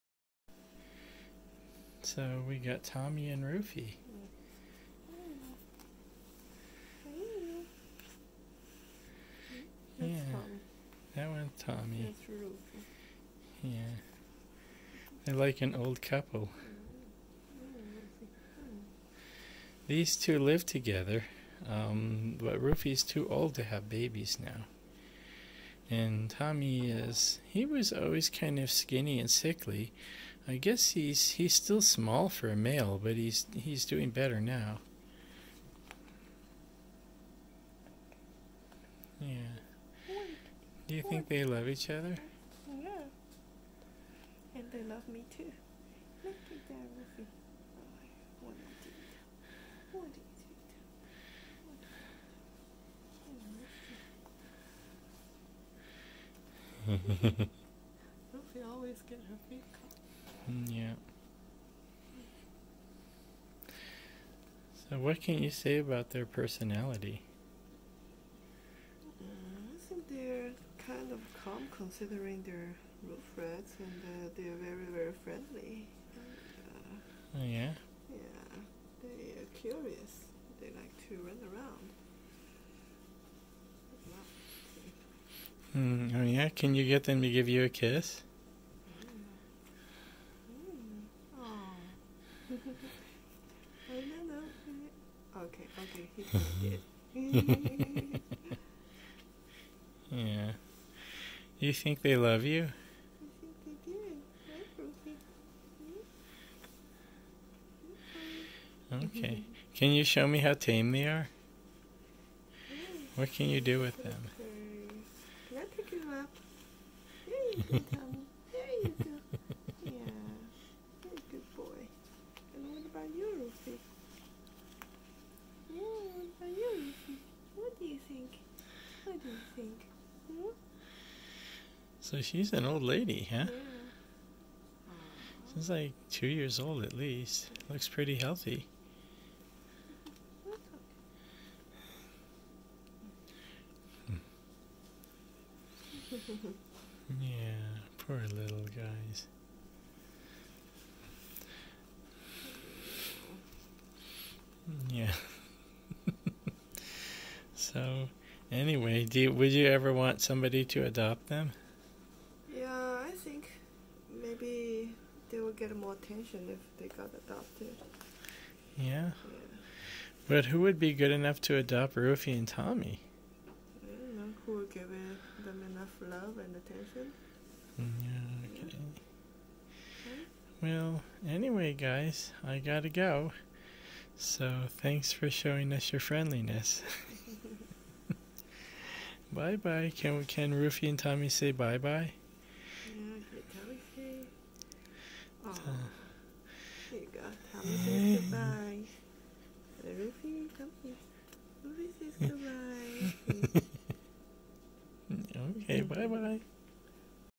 Roofy and Tommy are in a rattie May-September Relationship
Roofy is an old lady, Tommy is a young guy.  They got to know each other when Tommy was a sickly little runt.  I put him with Roofy because I knew she would be gentle and kind with him.

Tommy is doing much better now.  He's still a runt, and he's kind of slow.  But that's OK: so is Roofy, so they get along just fine!

And they are both the sweetest, friendliest and gentlest little Roof Rats ever.